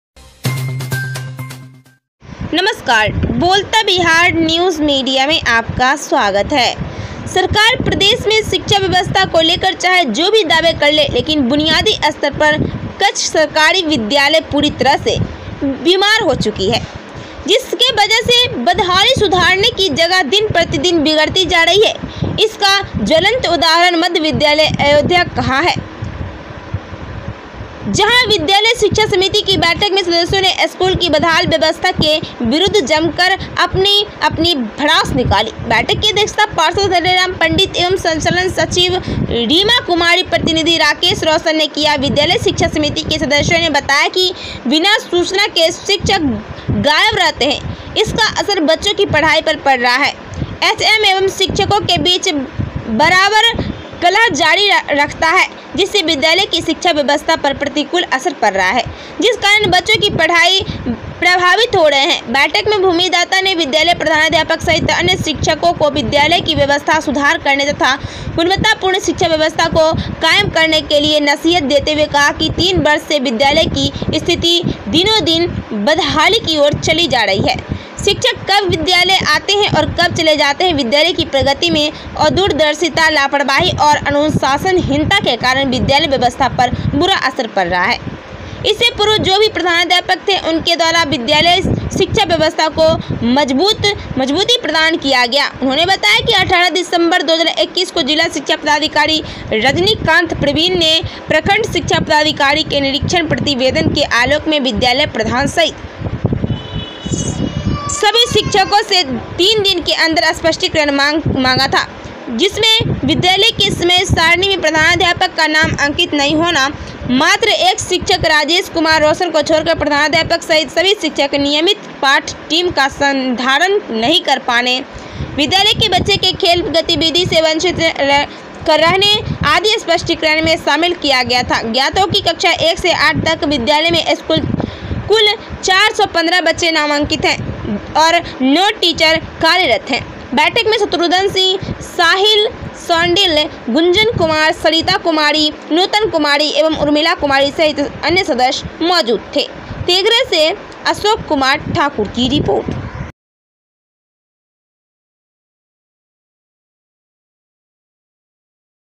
सरकारी विद्यालय की बदहाली चल रही है शिक्षकों की मनमानीbbnm
सरकारी विद्यालय की बदहाली चल रही है शिक्षकों की मनमानी।

देखभाल व उचित प्रबंधन के अभाव में विद्यालय की हालत बदतर
सरकार प्रदेश में शिक्षा व्यवस्था को लेकर चाहे जो भी दावे कर ले लेकिन बुनियादी स्तर पर कछ सरकारी विद्यालय पूरी तरह से बीमार हो चुकी है। जिसके वजह से बदहाली सुधारने की जगह दिन-प्रतिदिन बिगड़ती जा रही है।
इसका ज्वलंत उदाहरण मध्य विद्यालय अयोध्या का है। जहां विद्यालय शिक्षा  समिति की बैठक में सदस्यों ने स्कूल की बदहाल व्यवस्था के विरुद् जमकर अपनी अपनी भड़ास निकाली। बैठक की अध्यक्षता पार्षद हरे राम पंडित एवं संचालन सचिव रीमा कुमारी प्रतिनिधि राकेश रोशन ने किया।विद्यालय शिक्षा समिति के सदस्यों ने बताया कि बिना सूचना के शिक्षक गायब रहते हें। इसका असर बच्चों की पढ़ाई पर पड़ रहा है। एचएम एवं शिक्षकों के बीच बराबर कलह जारी रखता है । जिससे विद्यालय की शिक्षा व्यवस्था पर प्रतिकूल असर पड़ रहा है। जिस कारण बच्चों की पढ़ाई प्रभावित हो रहे हें। बैठक में भूमि दाता ने विद्यालय प्रधानाध्यापक सहित अन्य शिक्षकों को विद्यालय की व्यवस्था सुधार करने तथा गुणवत्तापूर्ण शिक्षा व्यवस्था को कायम करने के लिए नसीहत देते हुए कहा कि 3 वर्ष से विद्यालय की स्थिति दिनोंदिन बदहाली की ओर चली जा रही है।शिक्षक कब विद्यालय आते हें और कब चले जाते हें, विद्यालय की प्रगति में अदूरदर्शिता , लापरवाही और अनुशासनहीनता के कारण विद्यालय व्यवस्था पर बुरा असर पड़ रहा है। इससे पूर्व जो भी प्रधानाध्यापक थे उनके द्वारा विद्यालय शिक्षा व्यवस्था को मजबूती प्रदान किया गया। उन्होंने बताया कि 18 दिसंबर 2021 को जिला शिक्षा पदाधिकारी रजनीकांत प्रवीण ने प्रखंड शिक्षा पदाधिकारी के निरीक्षण प्रतिवेदन के आलोक में विद्यालय प्रधान सहित सभी शिक्षकों से 3 दिन के अंदर स्पष्टीकरण मांगा था जिसमें  विद्यालय   के समय सारणी में प्रधानाध्यापक का नाम अंकित नहीं होना, मात्र एक शिक्षक राजेश कुमार रोशन को छोड़कर प्रधानाध्यापक सहित सभी शिक्षक नियमित पाठ - टीम का संधारण नहीं कर पाने, विद्यालय के बच्चे के खेल गतिविधि से वंचित रहने आदि स्पष्टीकरण में शामिल किया गया था। ज्ञात हो कि कक्षा 1 से 8 तक विद्यालय में कुल 415 बच्चे नामांकित है और 9 टीचर कार्यरत हें। बैठक में शत्रुघन सिंह, साहिल शांडिल्य, गुंजन कुमार, सरिता कुमारी, नूतन कुमारी एवं उर्मिला कुमारी सहित सदस्य मौजूद थे